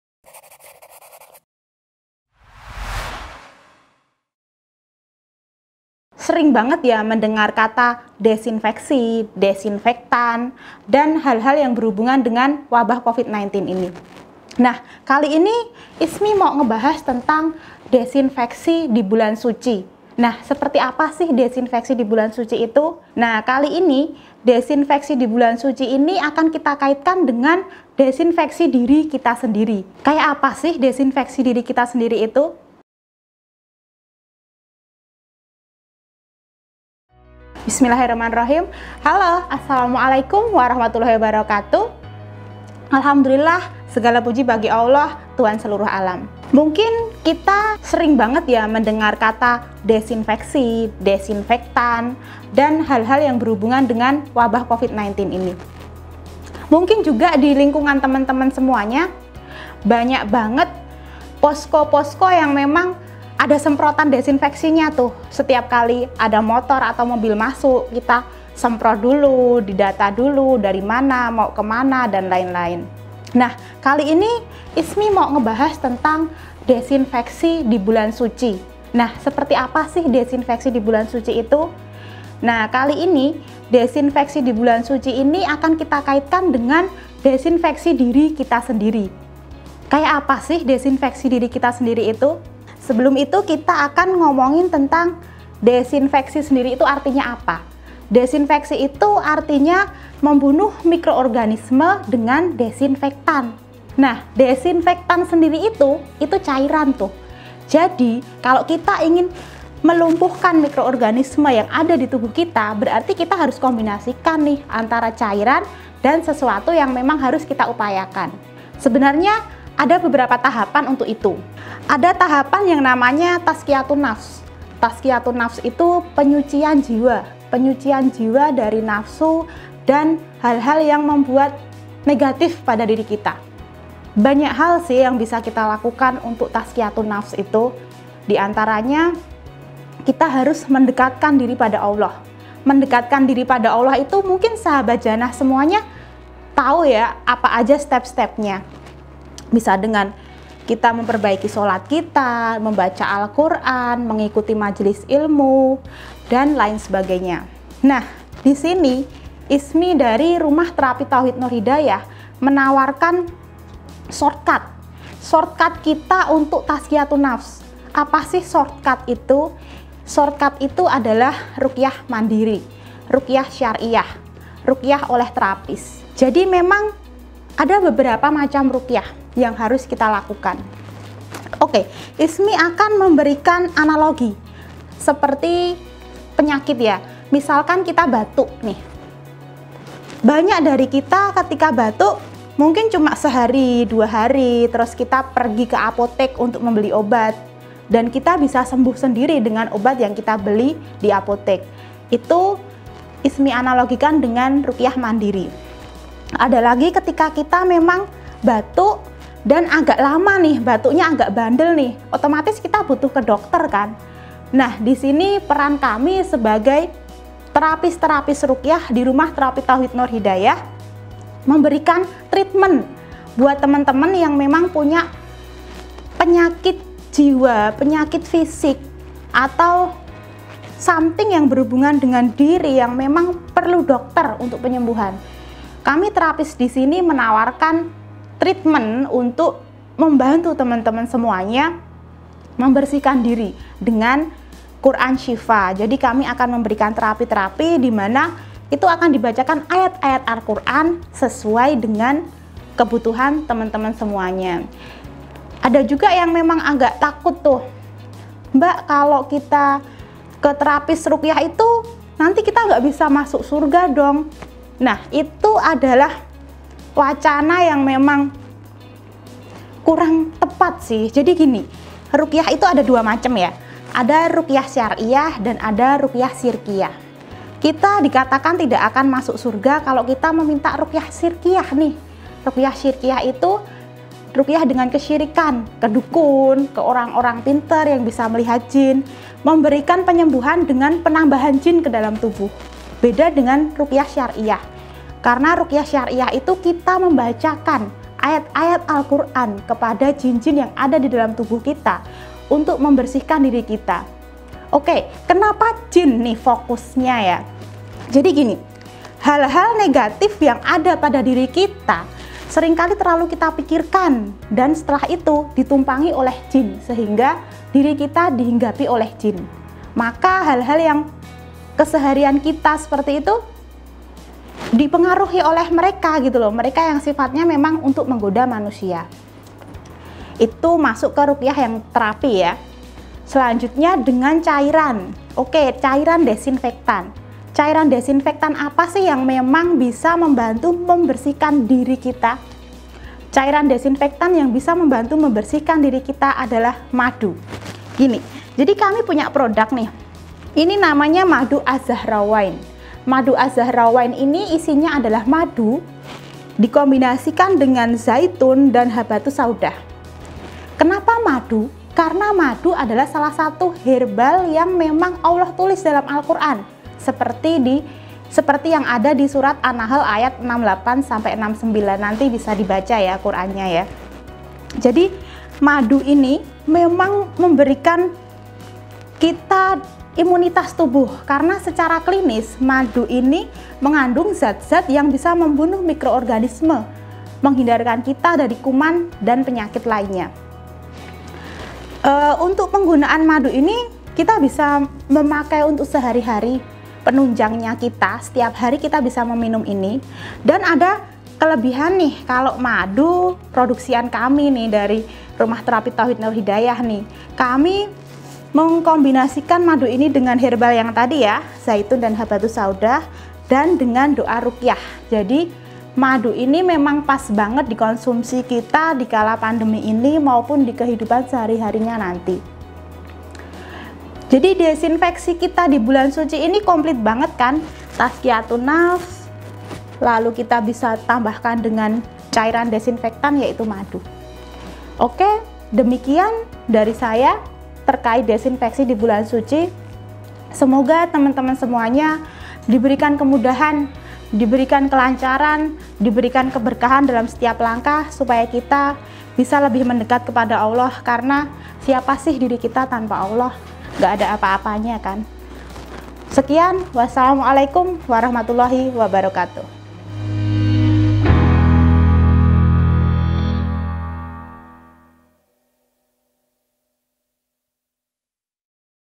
SERIAL NGAJI BARENG NUR HIDAYAH #19 (DISINFEKSI DI BULAN SUCI) bersama ISMI RAHAYU
❤️ DESINFEKSI di BULAN SUCI ❤️ ❁❁═❁❁═❁❁❁❁═❁❁═❁❁═❁❁

👩🏻‍🏫 Kita sering banget ya mendengar kata Desfinfeksi di era pandemi ini?

Sebenarnya apa sih desinfeksi itu?

Lalu apa arti desinfeksi di bulan suci?

Okay, kali ini kita akan membahas tentang desinfeksi yang dikaitkan dengan diri kita,

Pas banget juga dengan momen Ramadhan ini,

Desinfeksi ini bisa kita lakukan dengan beberapa tahap, yaitu 👇🏻

🌿Tazkiyatun Nafs
🌿Ditunjang cairan Desinfektan untuk diri kita

Wahh wahh kita pakai cairan desinfektan?? 😨

Yang bener aja? 😱

Eitss, tunggu dulu... Biar ga salah paham,,

👉🏻 Segera Klik Link Video Youtube ini untuk penjabaran dari Tema kita hari ke-19 Ramadhan

Semoga bermanfaat..